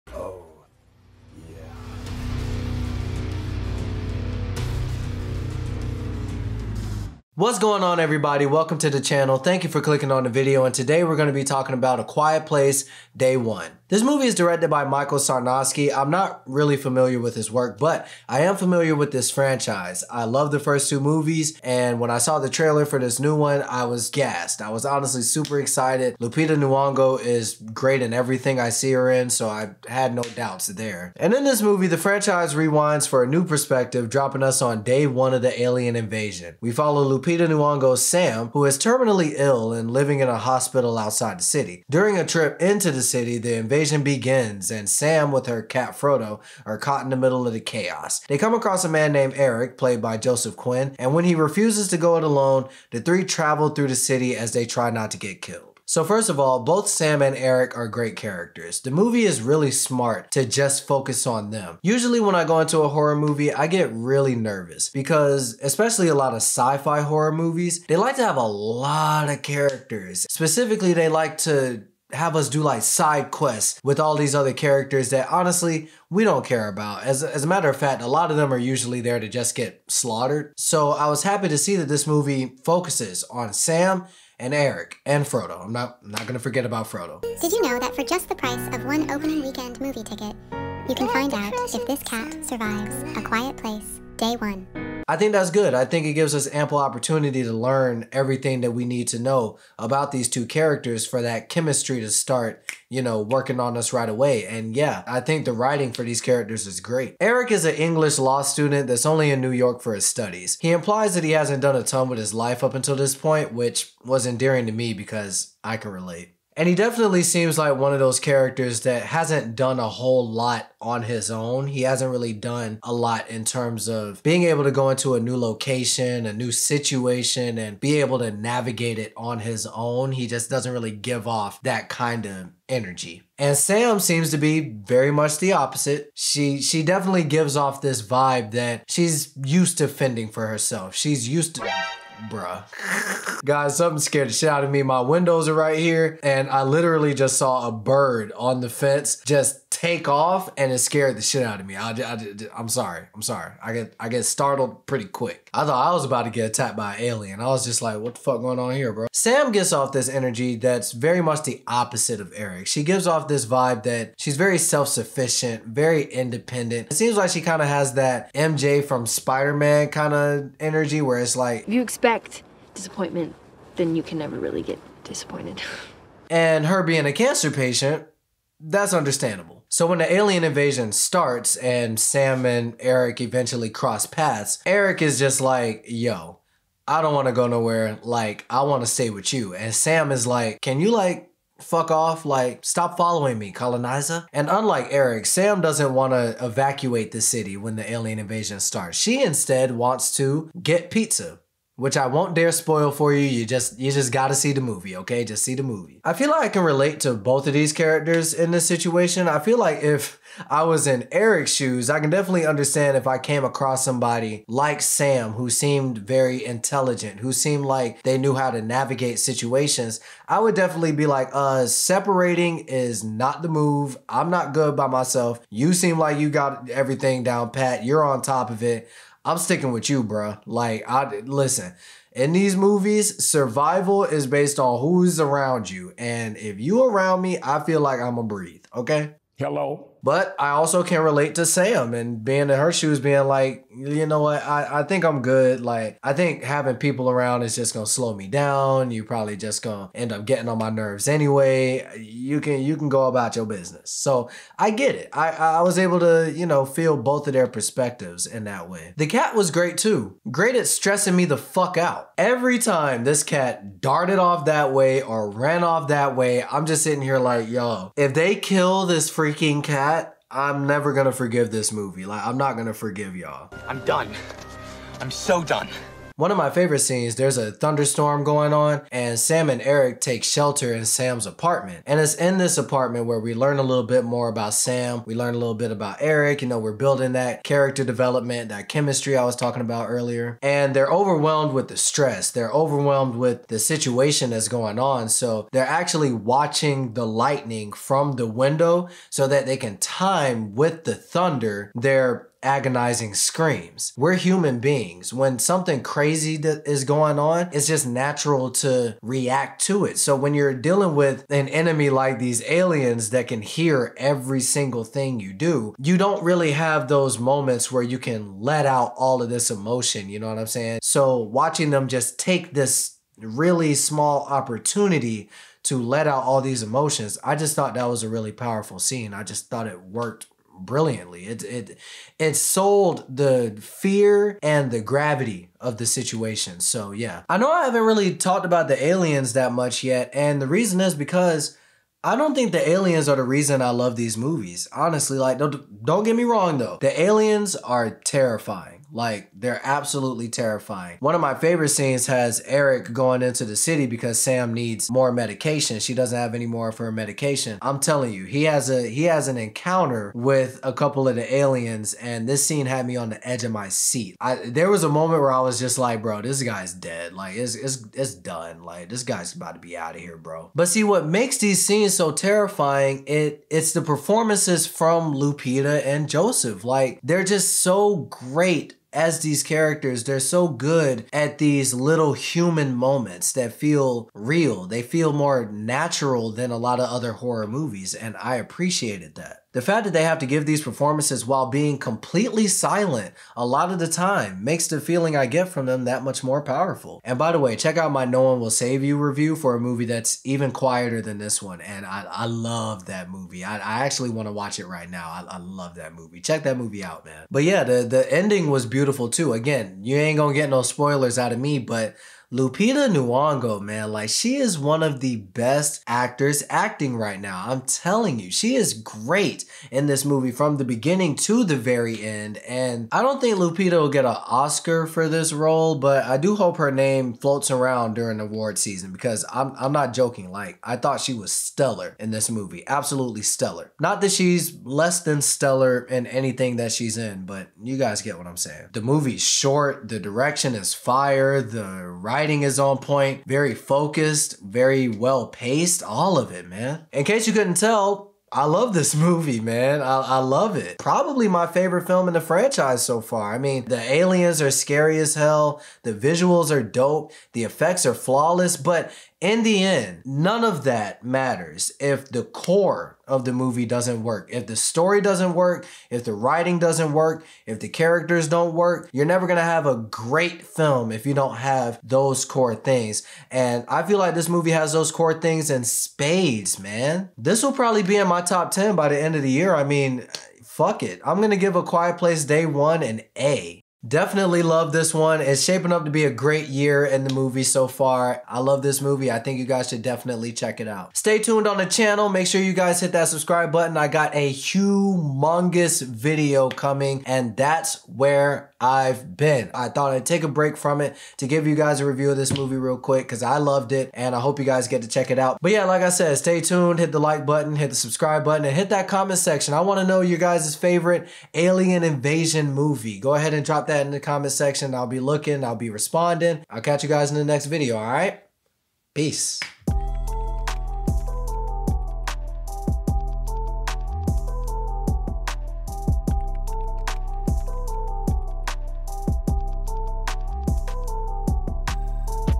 A Quiet Place: Day One - Movie Review!!
With this new installment in the franchise, A Quiet Place proves yet again that you don't have to sacrifice quality storytelling and characters to have an effective alien invasion horror flick!